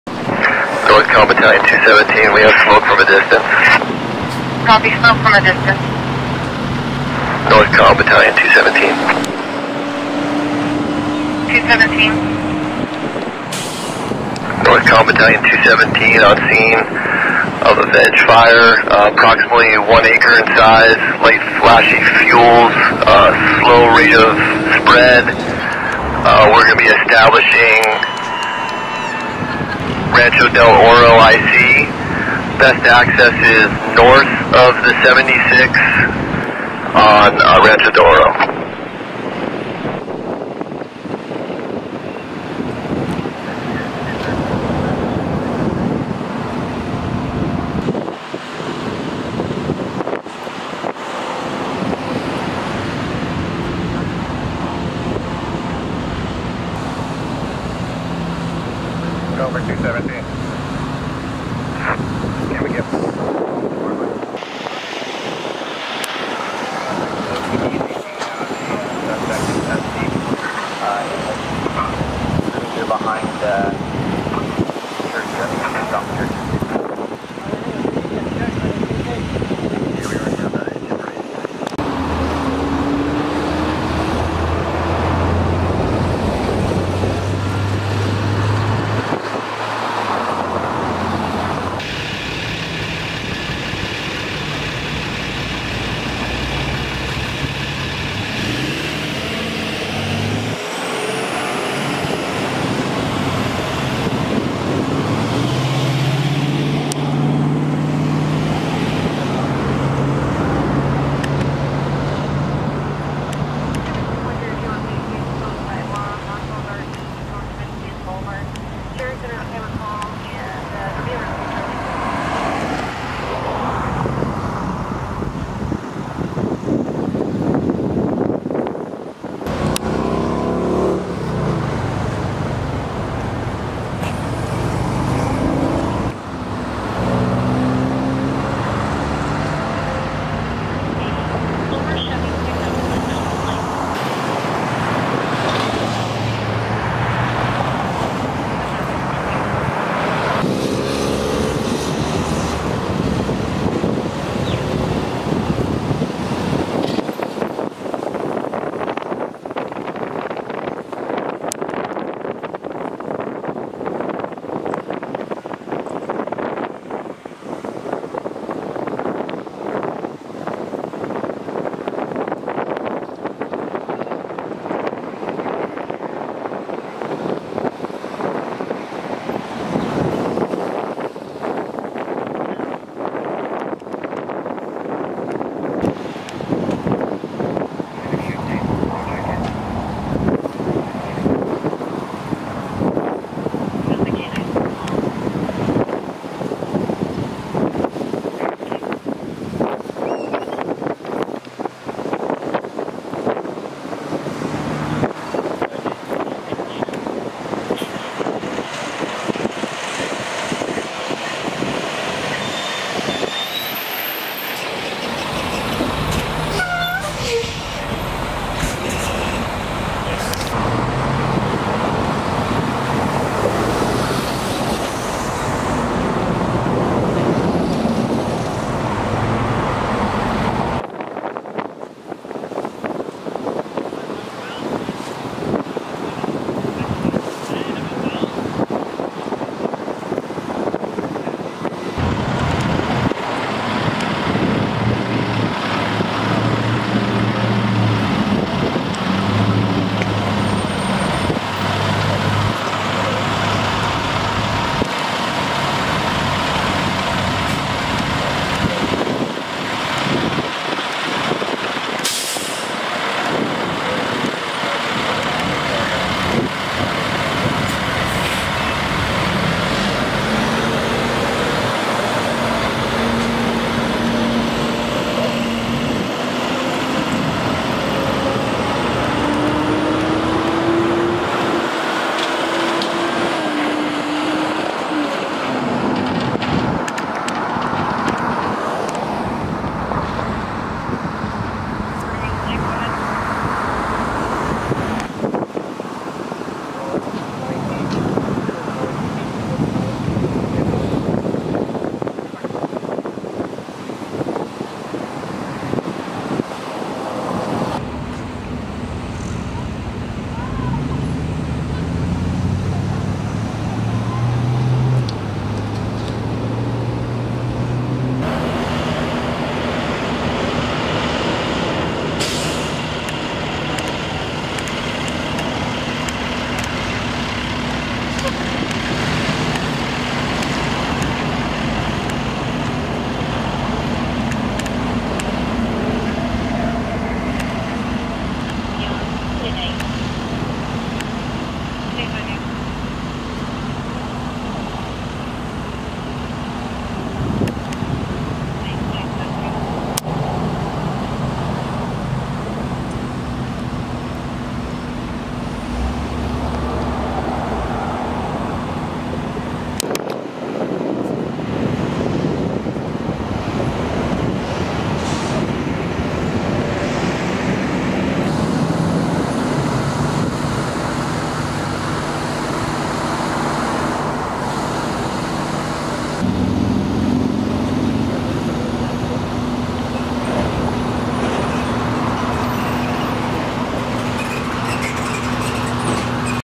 Suspect In Custody After Starting Brush Fire (Oceanside)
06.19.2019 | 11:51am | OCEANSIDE

At 11:51am, North Comm dispatched Oceanside Fire Department and Camp Pendleton Fire Department units to a Vegetation Fire near eastbound lanes of SR-76 and Rancho Del Oro Drive in Oceanside. Battalion 217 arrived on scene and reported a vegetation fire, "approximately 1 acre in size, light flashy fuels, slow rate of spread" and established Rancho Del Oro (RDO) IC. A quick and aggressive fire attack was made by firefighters which kept the fire from spreading to adjacent structures and further into the bordering cemetery.

Rancho Del Oro IC reported that the fire was fully knocked down and contained at 12:46pm, and all units were able to go available within 83 minutes of the initial dispatch.

No structures were damaged.

Oceanside Police Department reported they had one suspect in custody in relation to the brush fire.